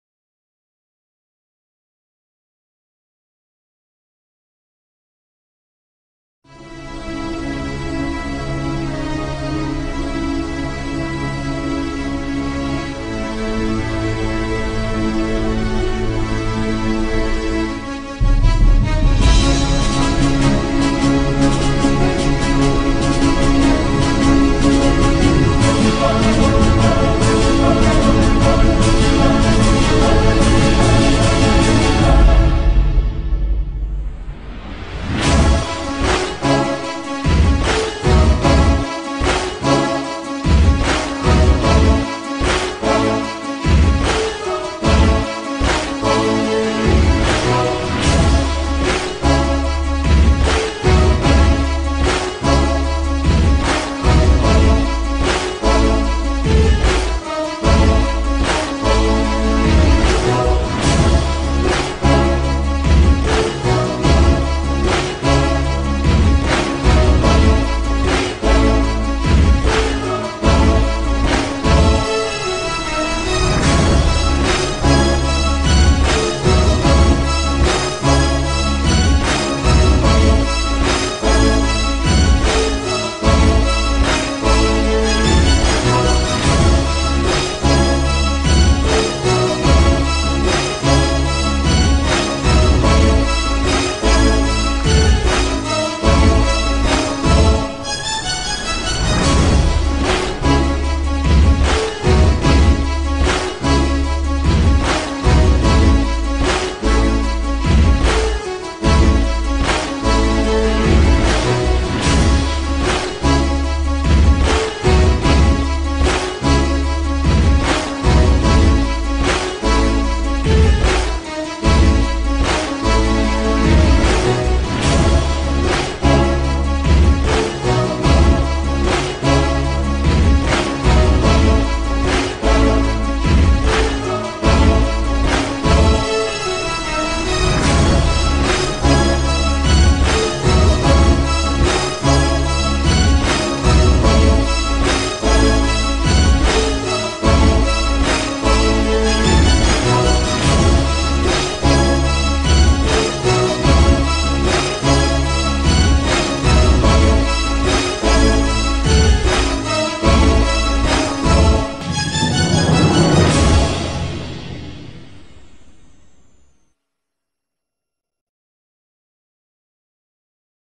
Presentazione Scandone - IrpiniaReport
omaggio di igor capossela alla scandone e a i tifosi biancoverdi